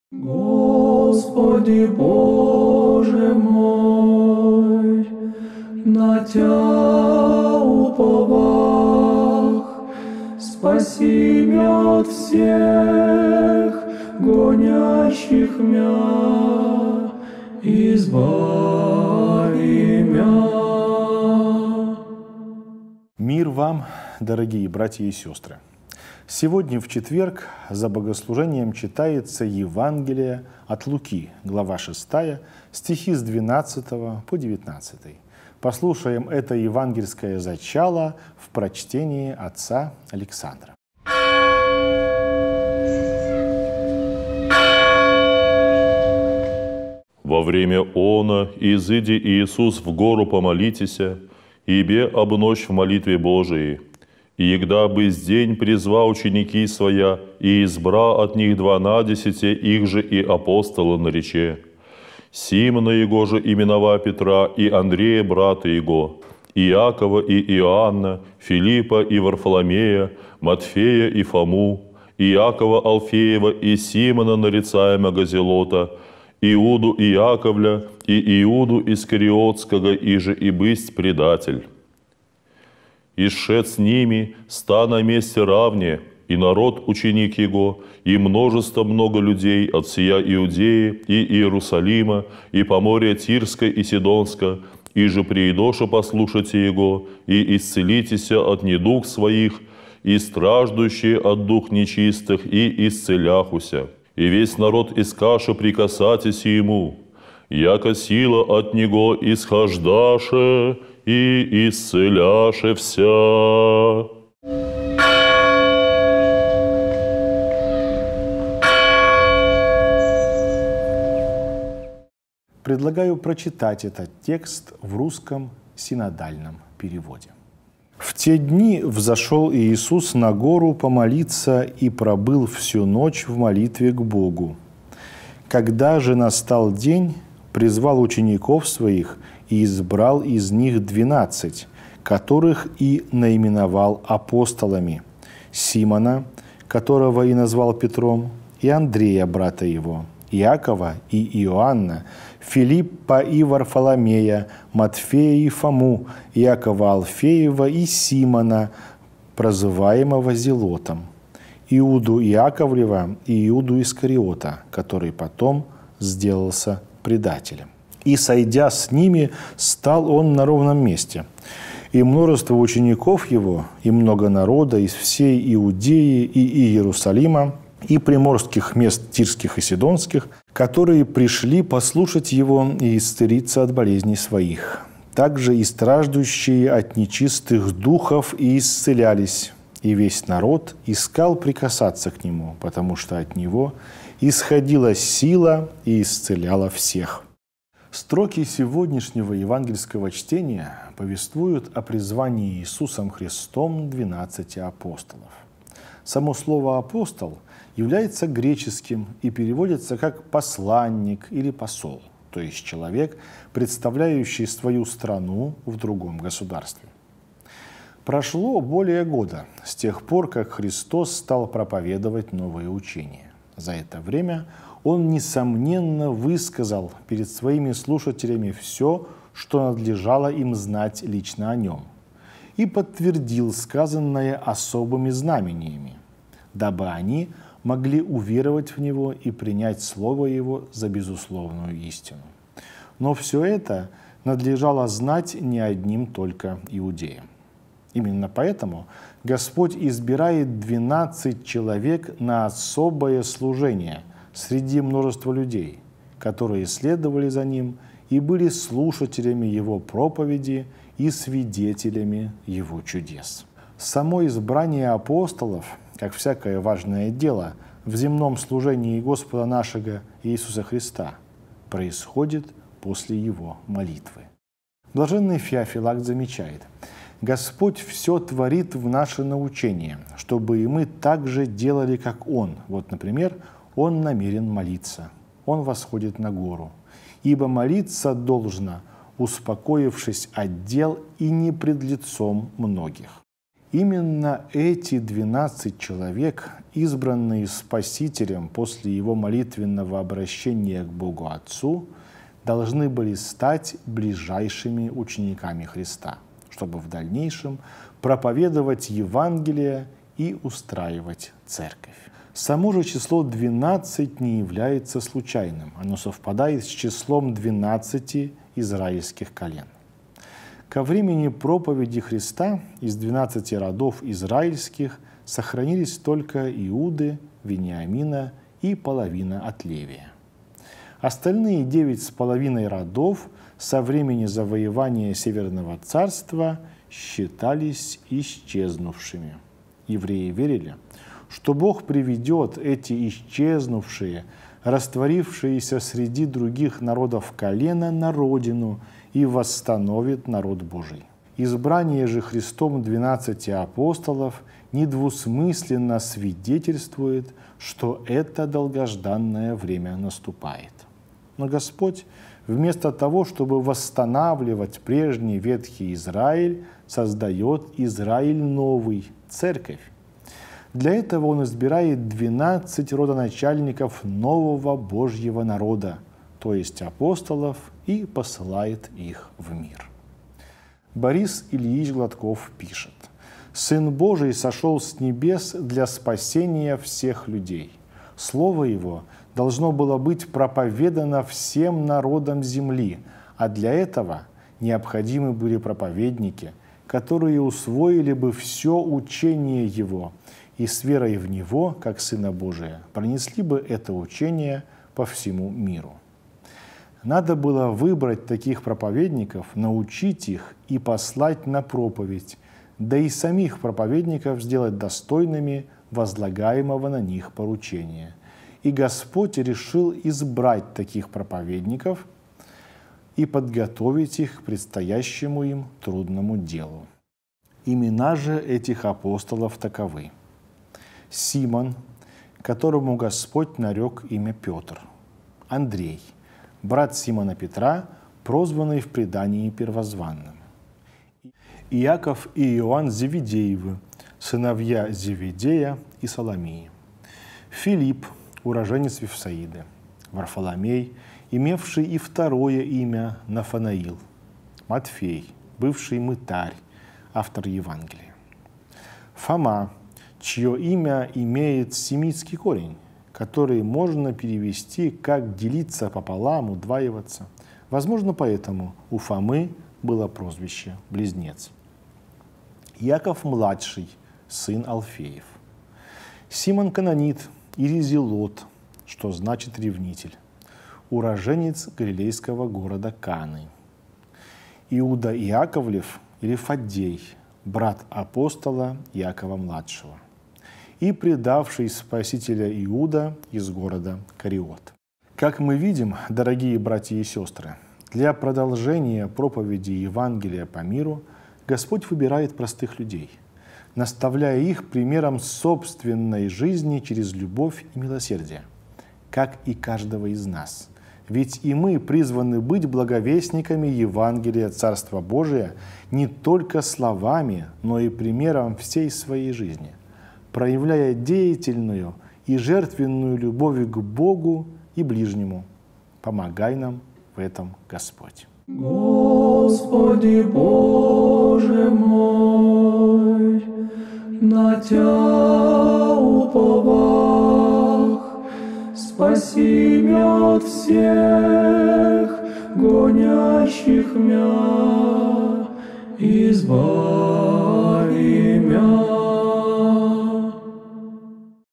Читаем Евангелие 10 октября 2024. Евангелие от Луки. Глава 6, ст. 12-19.
И весь народ искал прикасаться к Нему, потому что от Него исходила сила и исцеляла всех
Видеоматериал используется из источника телеканала «Союз»
Святоотеческое толкование Библии: